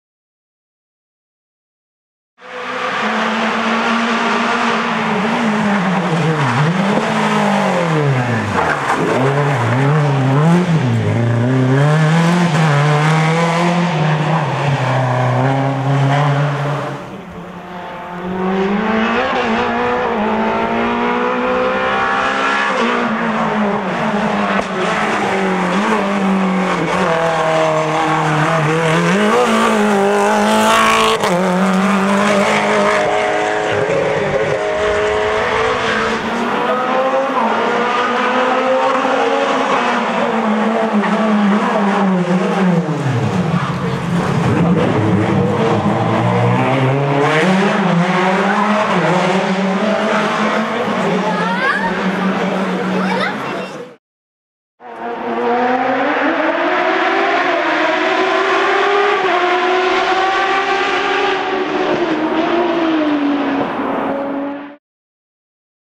Renault Clio 1 16S - Guillaume ANCEL - RALLY - 2020 - Centre Alsace + Rémy SCHMITT
Rallye du Centre-Alsace 2020
Rally : France - Alsace – Bas-Rhin - Haut-Rhin - Rallye du Centre-Alsace 2020 (N°6 (Historic N°6, VHRS N°5))